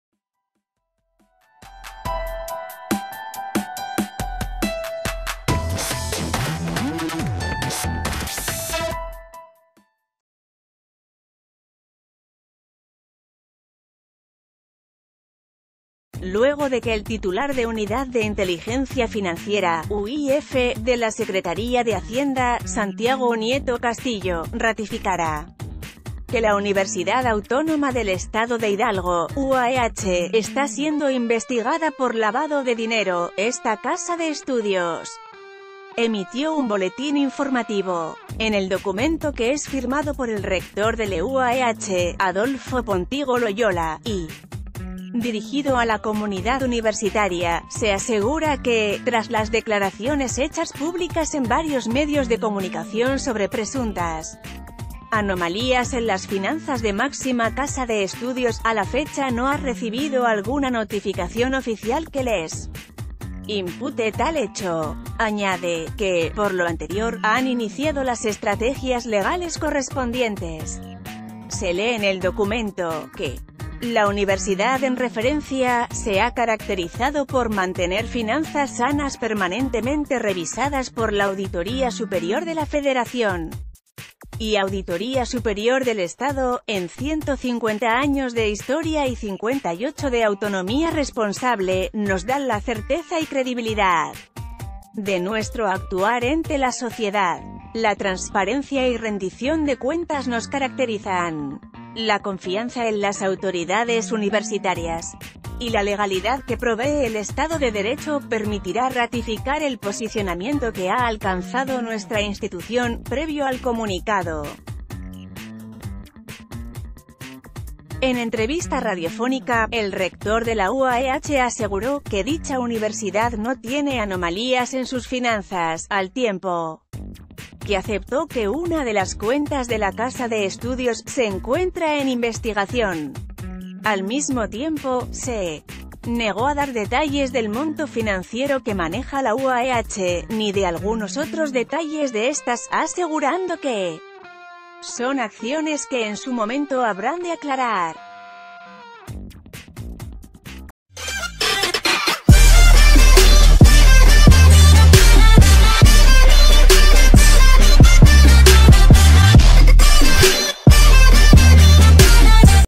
UAEH acepta que una cuenta bancaria se encuentra en investigación
Luego de que el titular de Unidad de Inteligencia Financiera (UIF) de la Secretaría de Hacienda, Santiago Nieto Castillo, ratificara que la Universidad Autónoma del Estado de Hidalgo (UAEH) está siendo investigada por lavado de dinero, esta casa de estudios emitió un boletín informativo. En el docum...